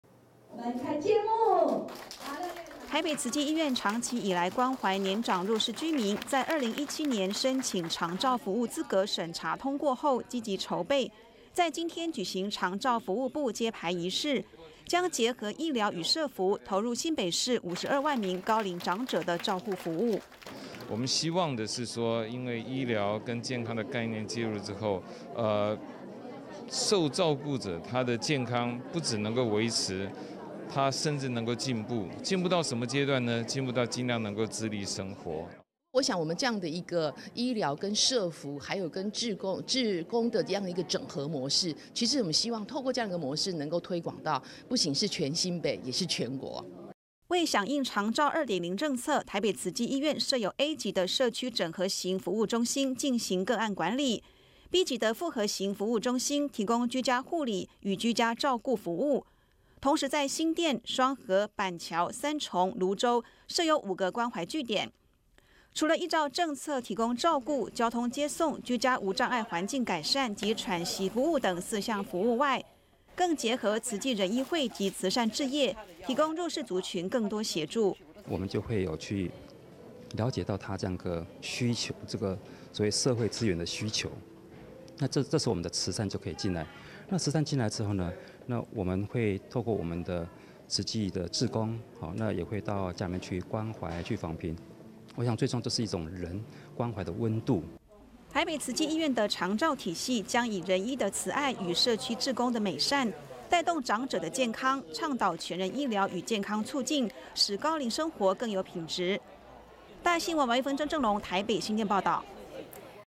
台北慈濟醫院 長照服務部揭牌
長照2.0，醫療邁大步。台北慈濟醫院，９月加入長照2.0體系，今天舉行長照服務部，揭牌儀式，是新北市少數同時具有A、B級、並結合社區據點服務的醫療機構。在這背後，包括人醫會及慈善志業，志工人力投入，帶動長者的健康。

台北慈濟醫院長期以來關懷年長弱勢居民，在2017年申請長照服務資格審查通過後積極籌備，於今天舉行長照服務部揭牌儀式，將結合醫療與社服，投入新北市５２萬名高齡長者的照護服務。

為響應長照2.0政策，台北慈濟醫院設有Ａ級的社區整合型服務中心進行個案管理、Ｂ級的複合型服務中心，提供「居家護理與」與「居家照顧服務」，同時在新店、雙和、板橋、三重、蘆洲設有五關懷據點。

除了依照政策提供照顧、交通接送、居家無障礙環境改善及喘息服務等四項服務外，更結合慈濟人醫會及慈善志業，提供弱勢族群更多協助。

台北濟醫院的長照體系，將以人醫的慈愛與社區志工的美善，帶動長者的健康，倡導全人醫療與健康促進，使高齡生活有品質。

王鈺棻 張正隆 台北新店報導